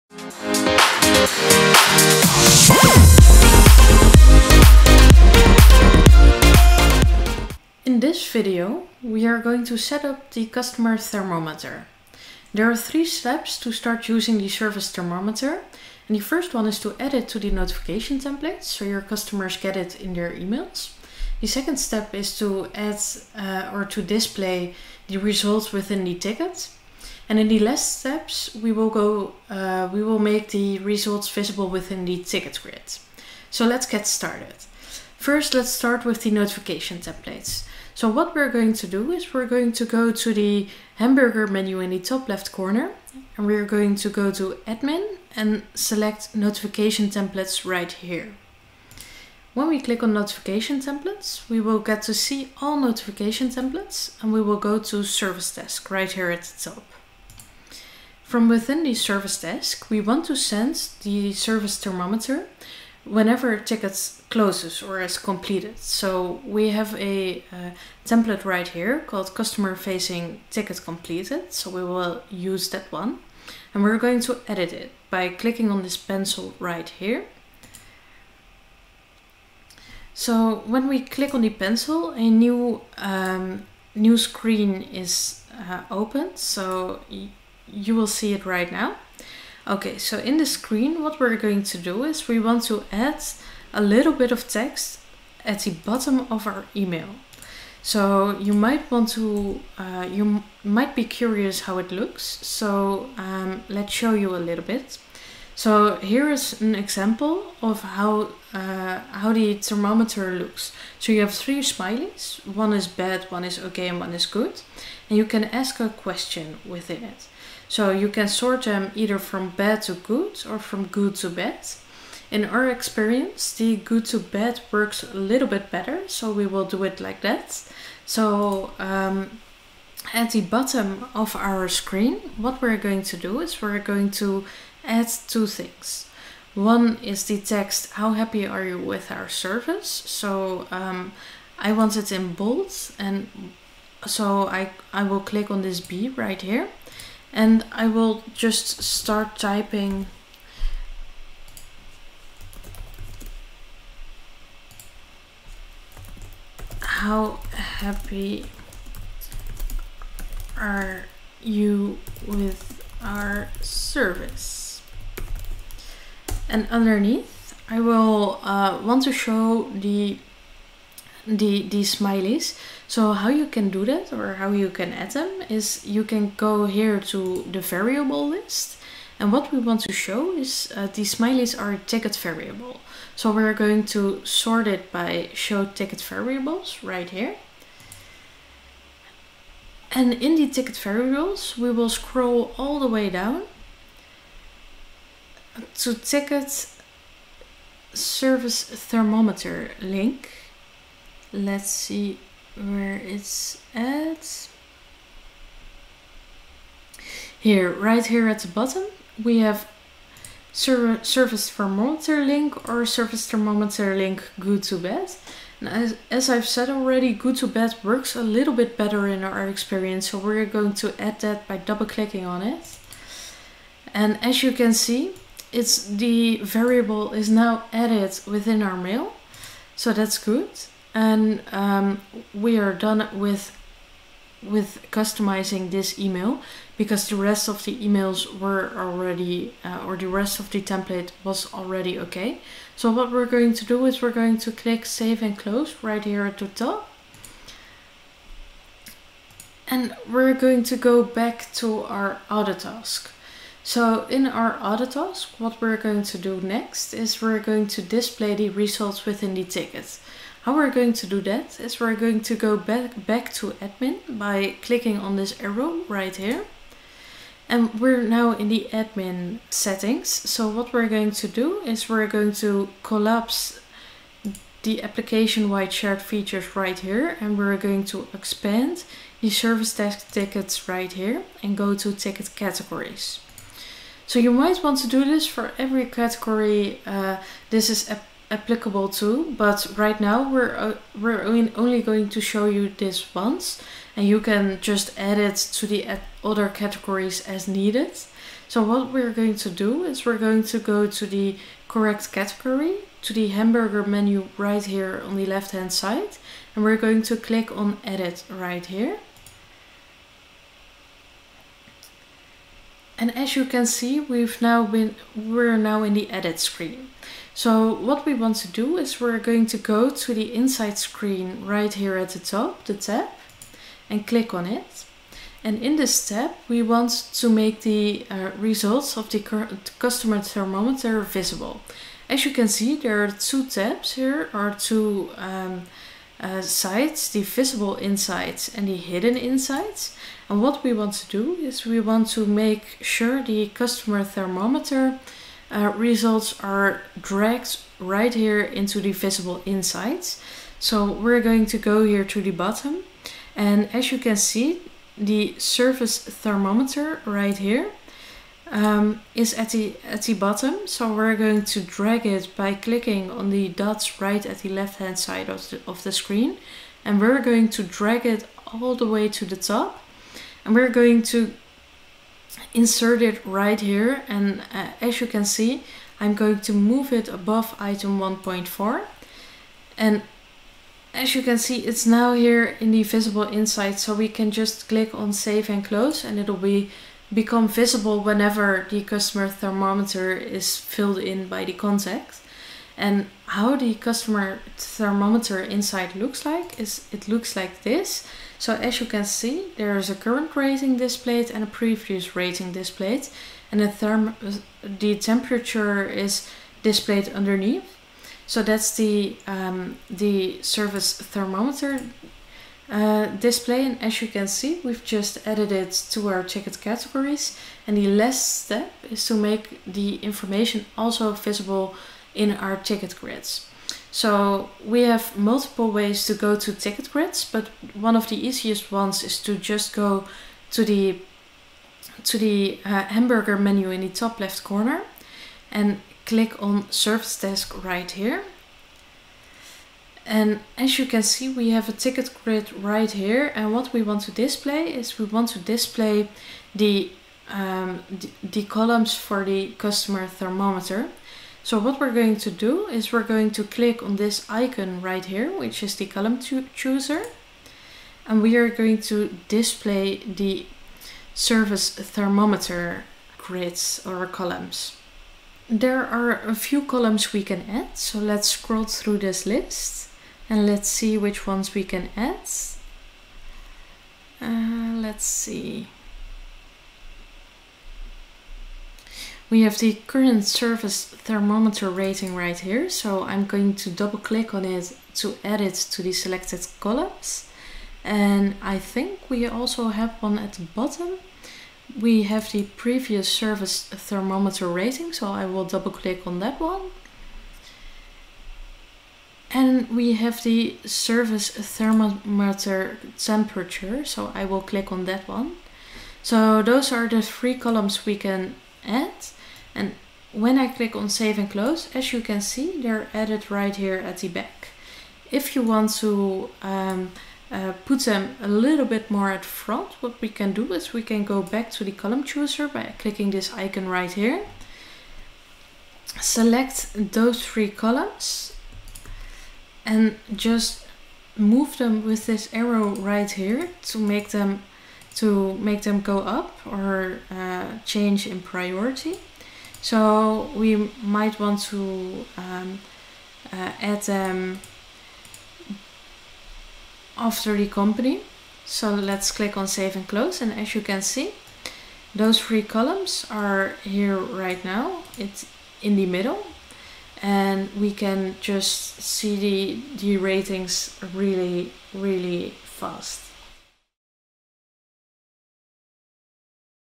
Dxfferent | Autotask Bootcamp Course - 5.06 Customer Thermometer
Essentially the customer thermometer is a way to manage (and manage how you manage) your KPIs in terms of customer interaction and satisfaction. In this lesson, we will cover all of the different ways that your customer thermometer can be viewed and manage to keep track of customer satisfaction.